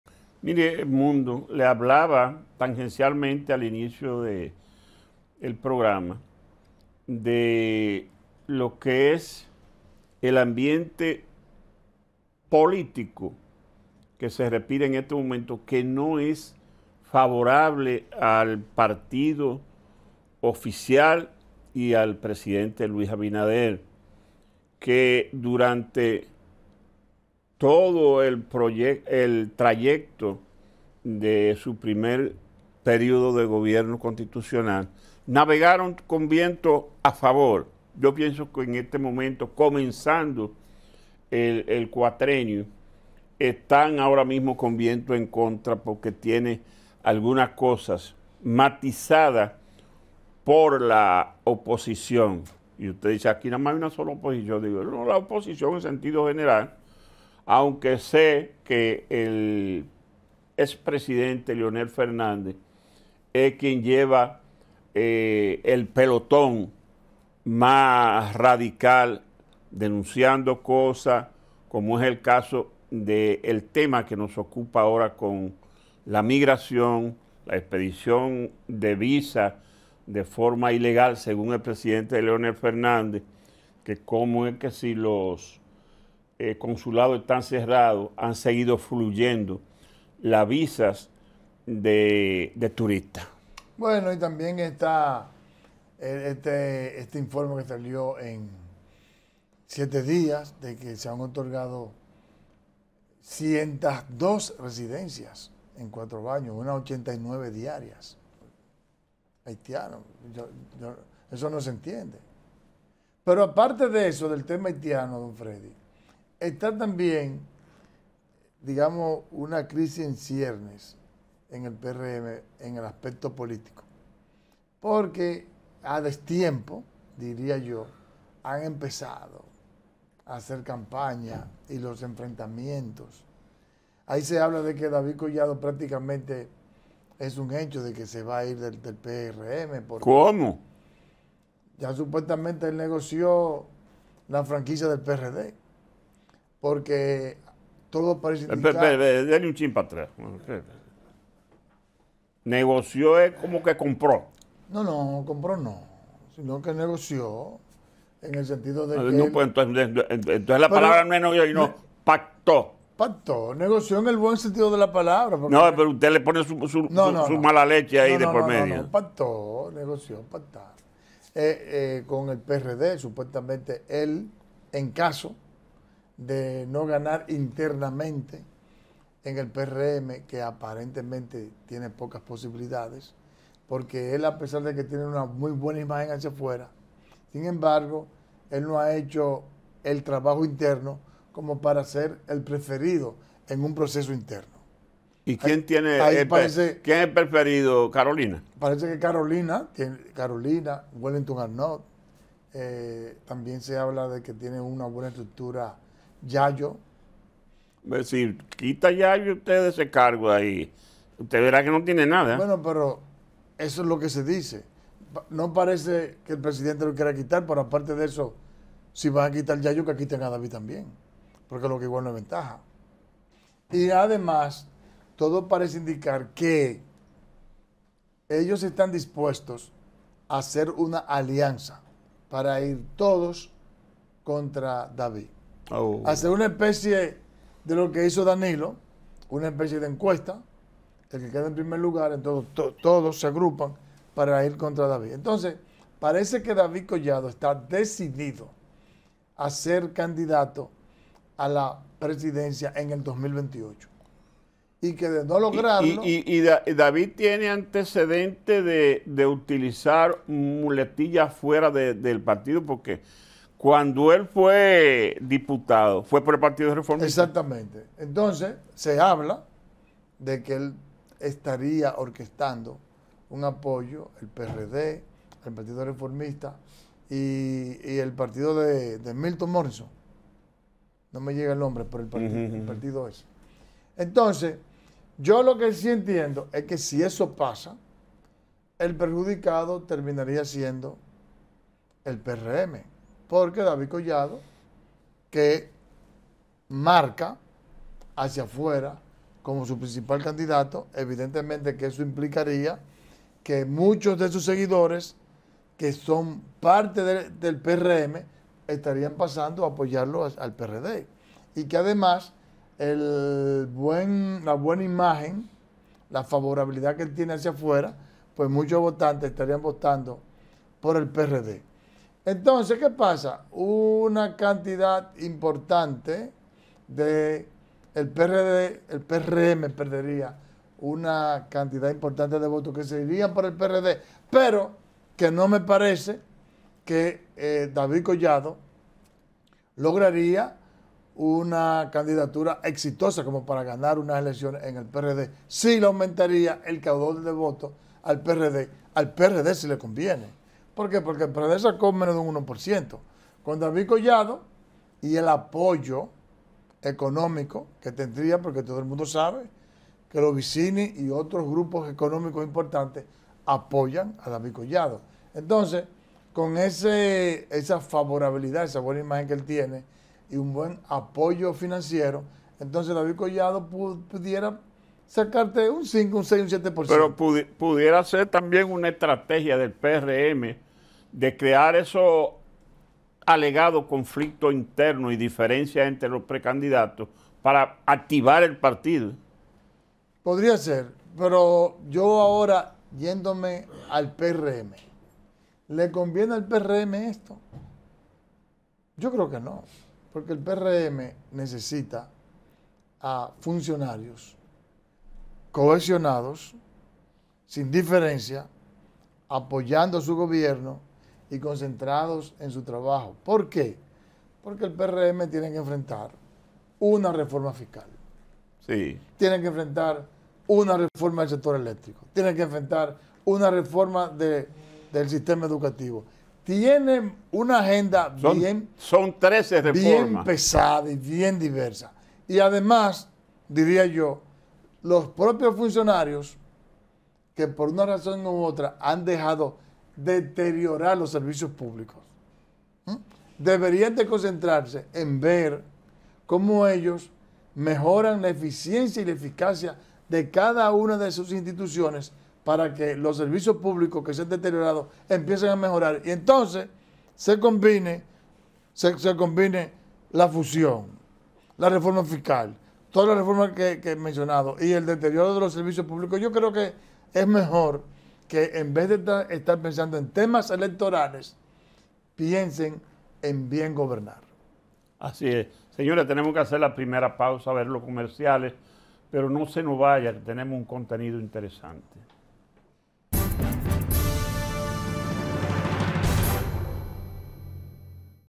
David Collado se va del PRM si pierde elecciones internas, se va para PRD
PRM rumbo a una crisis interna. Debe manejar crisis  socioeconómica en ciernes debido al colapso servicios públicos,  las promesas incumplidas y las reformas pendientes, en medio de pugnas internas